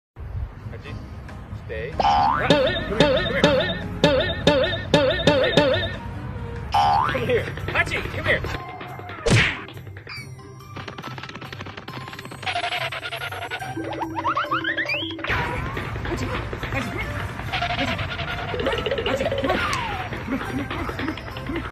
Trying to train a husky in zoomie mood 😂
Huskies do the best zoomies 😂